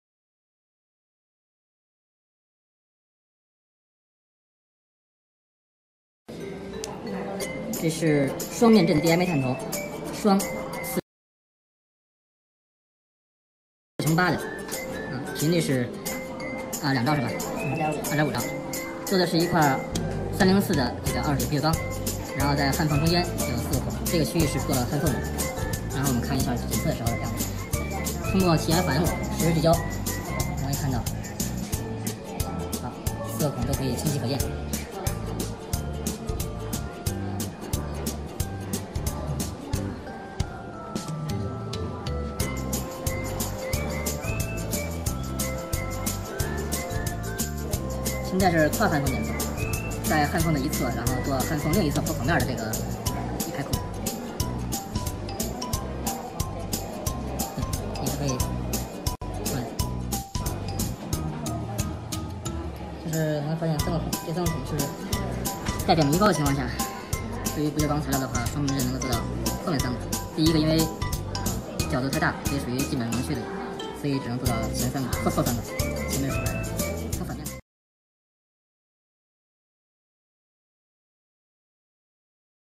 ISONIC 2009 / PAUT / DMA Probe / Stainless Steel Weld / FMC/TFM Imaging
ISONIC 2009 / PAUT - Use of Dual Matrix Array (DMA) Probe on the Customer's Sample of Stainless Steel Weld - Chinese National NDT Conference - June 2019 / Expert DMA Inspection SW Application:
True-to-Geometry (True-to-Shape) S-Scan Coverage / Toggling to Live FMC/TFM Imaging